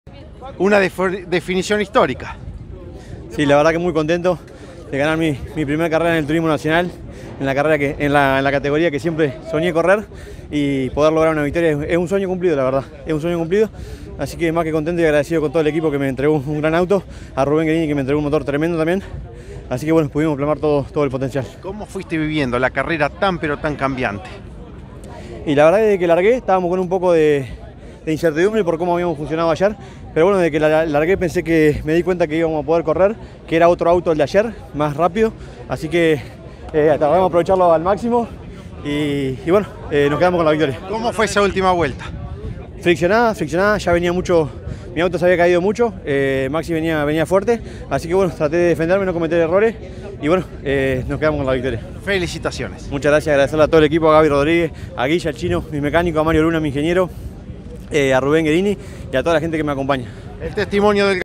TN Clase 2: Marco Veronesi logró un triunfo milimétrico
En Trelew, el entrerriano le ganó por seis milésimas a Bestani.